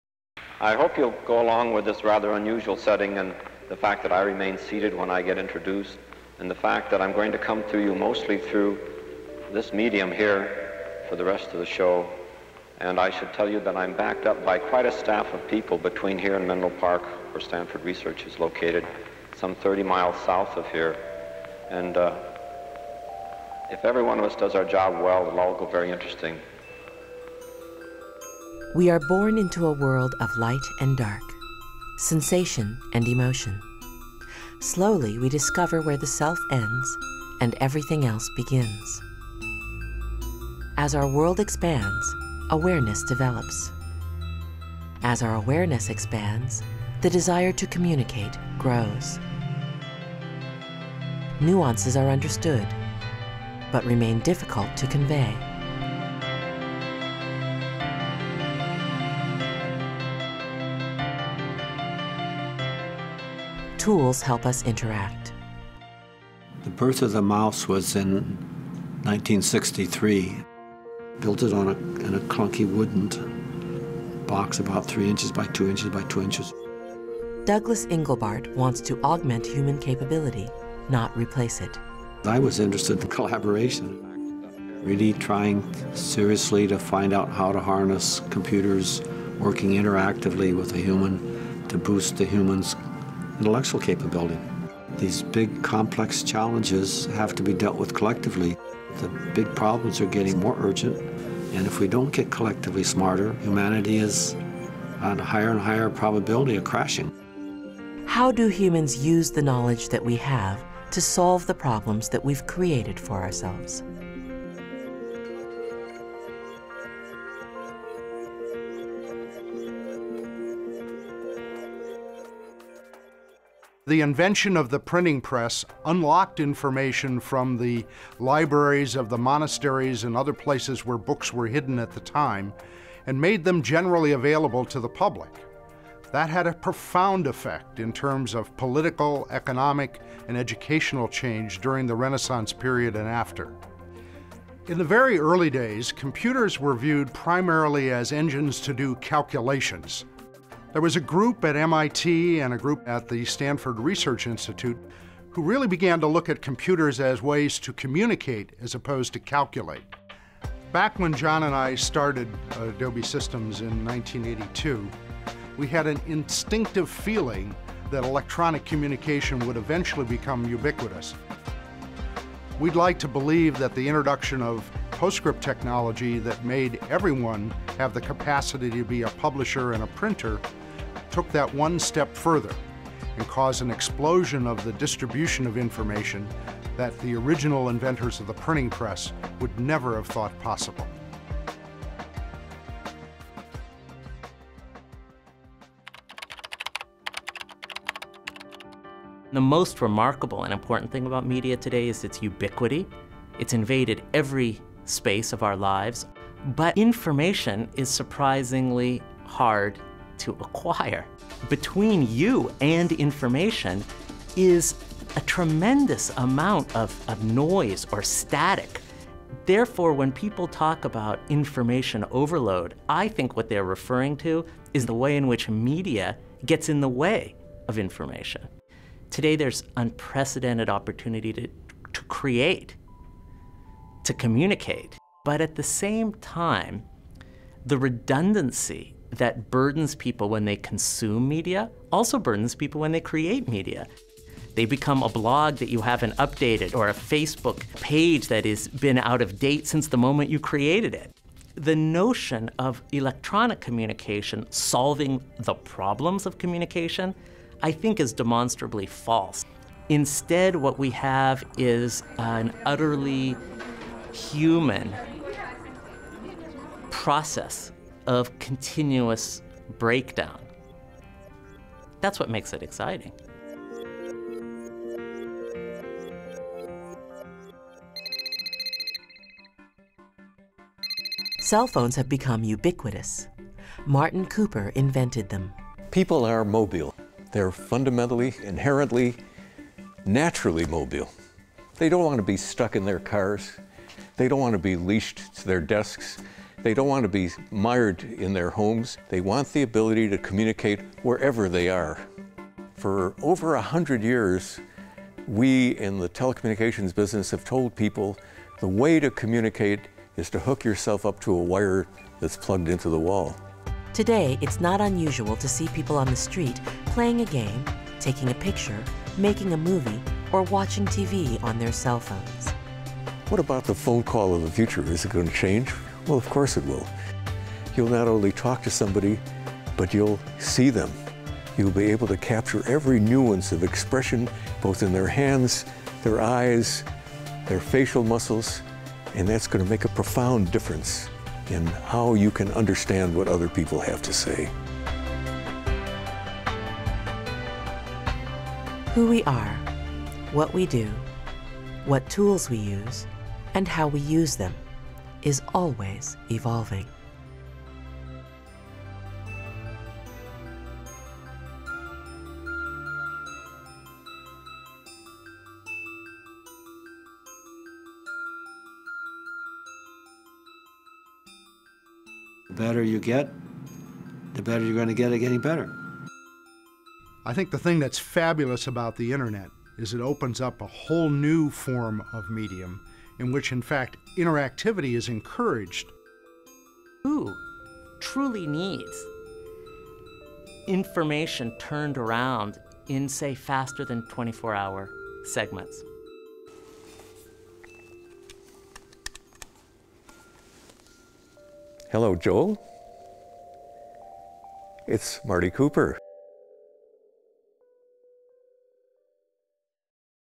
Evolution of Communication--Adobe Systems
Adobe Systems create a film depicting the Evolution of Communication. Various luminaries such as Doug Englebart (inventor of the mouse) and Martin Cooper (inventor of the cell phone) helped tell the story of how communication has changed and transformed over the past two decades. The film supported Adobes CEO in his keynote address. The production follows the arc of human communication from the moment when we are able to discern shapes and sounds to the point of complex interactions as adults. The story leads to a hint of where we are heading in the future, and to Adobes commitment to facilitate personal and human communication and connection.

Written and Produced by Robert Lundahl.